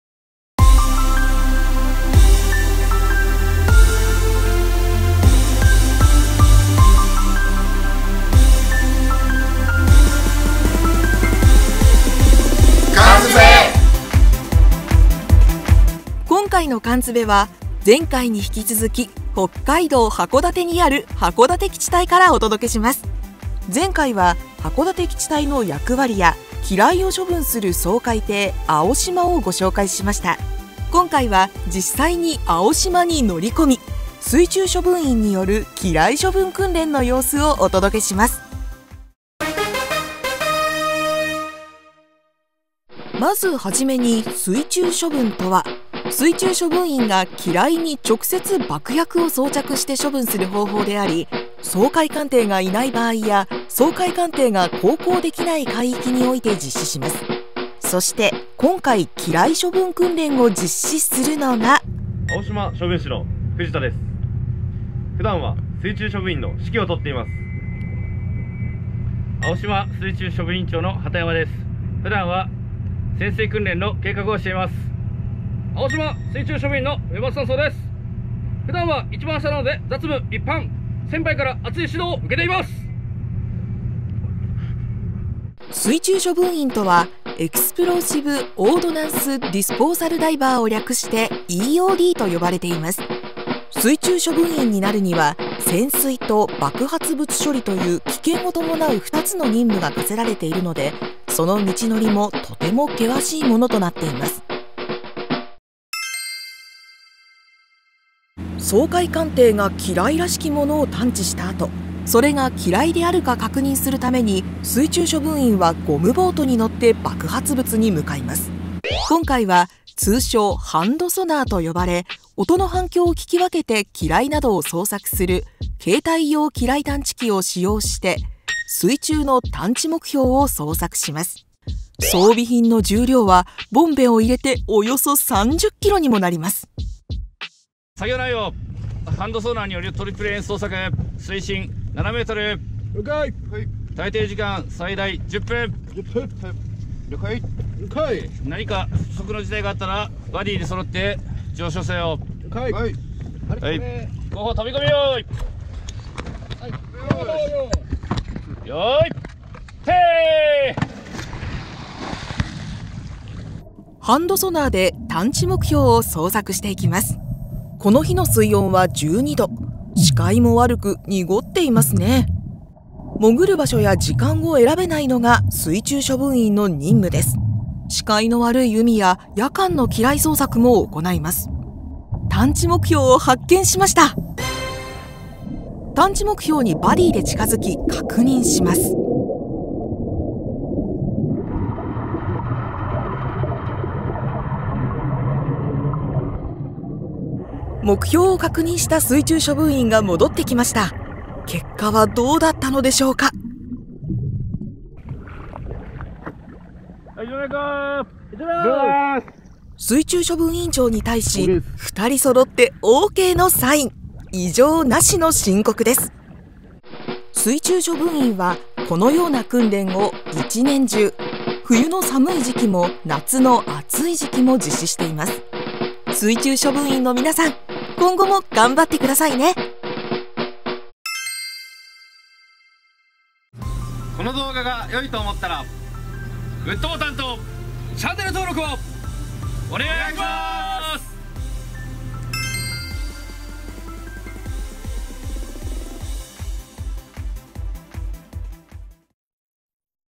【艦Tube】「水中処分員の機雷処分訓練に同行してみた！」
今回の艦Tubeも、前回に引き続き北海道函館基地隊からお届けします！
水中処分員とは？日頃どんな訓練をするの？そんな疑問を抱いたそこのあなた！この動画をぜひご覧ください！